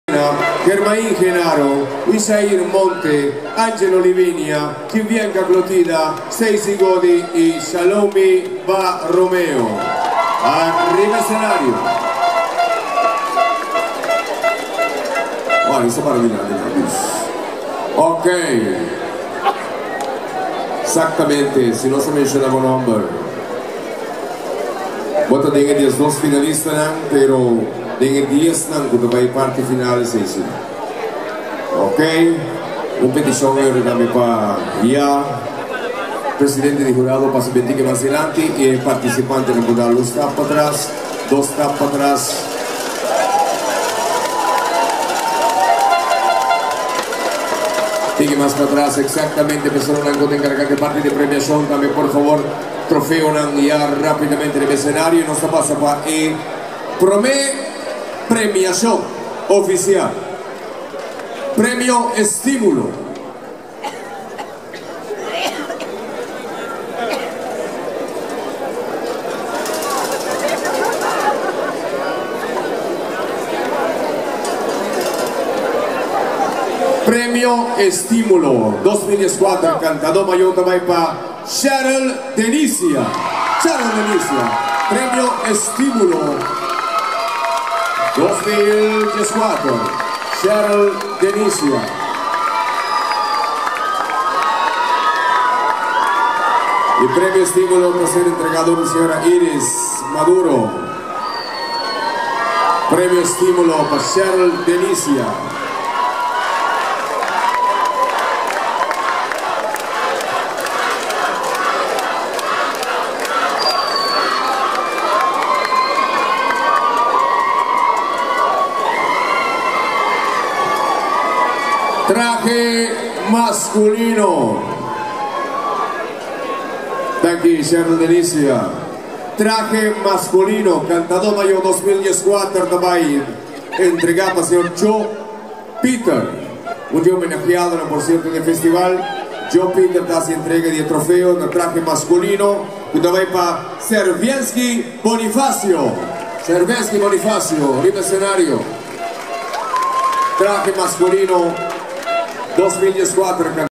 Seú 2014, Kantadó Mayó final 15
Harvest Festival Music Competition at Club di Shell 20140329.
Captured by Joscelin Trouwborst with Lumix DMC-TZ30.